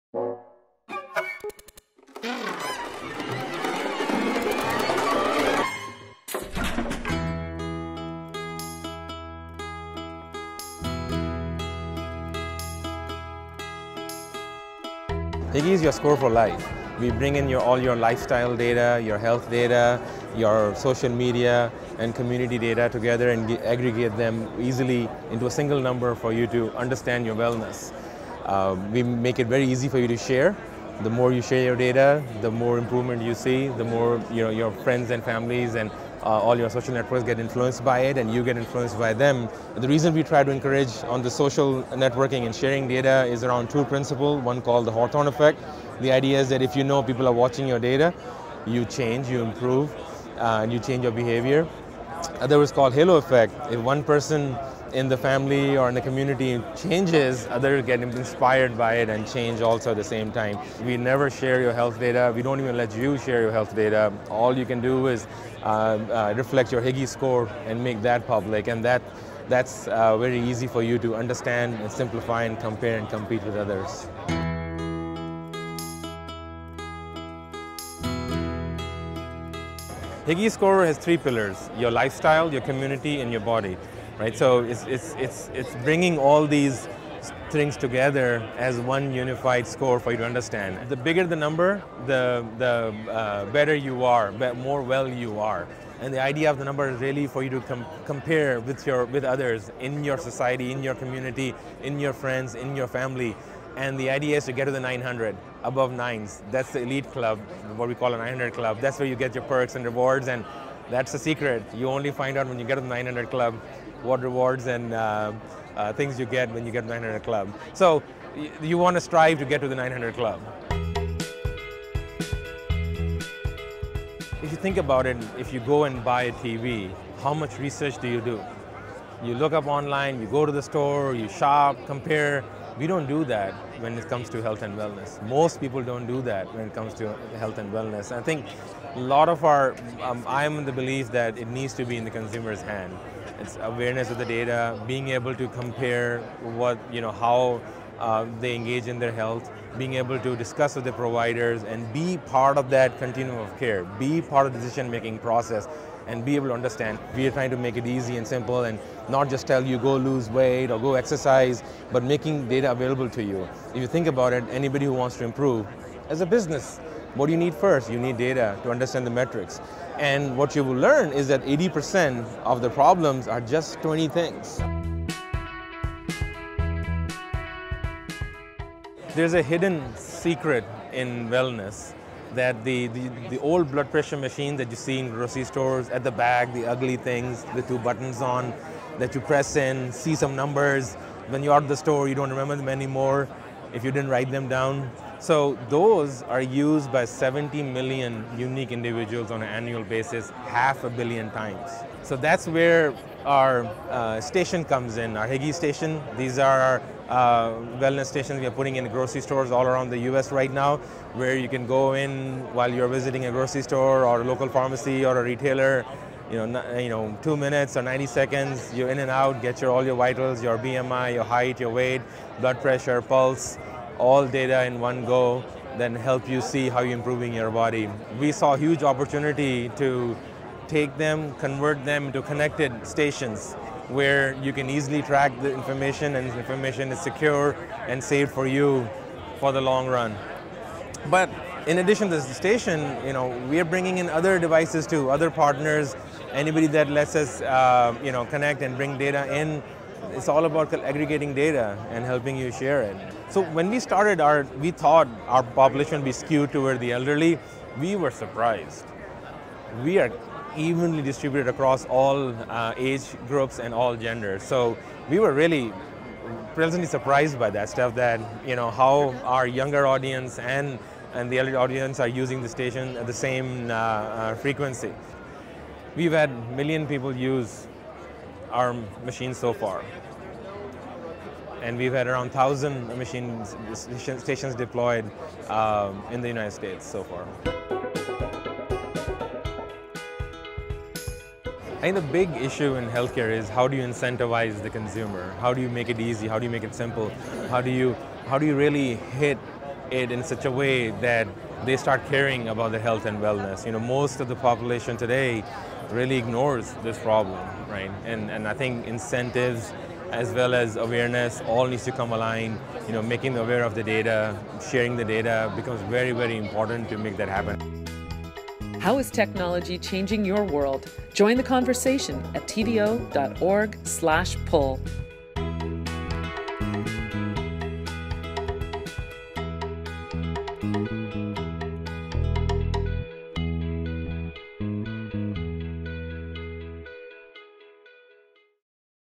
Dr. Khan Siddiqui: Incentives and competition can activate healthy behaviour
Social networking and sharing health data can change people's behaviour for the better according to Dr. Khan Siddiqui, the CEO of higi llc. The company provides a website, iPhone app, and wellness stations at pharmacies and grocery stores. The stations track your "higi Score For Life", a lifestyle and health score out of 999. Encouraging people to share their score with their social networks creates friendly competition but doesn't divulge the specifics of their weight, blood pressure, BMI or pulse. Once people reach the 900s in their score, they receive rewards and perks.

A radiologist, engineer and entrepreneur, Siddiqui also spent time as an executive with Microsoft before embracing social gamification in health and wellness.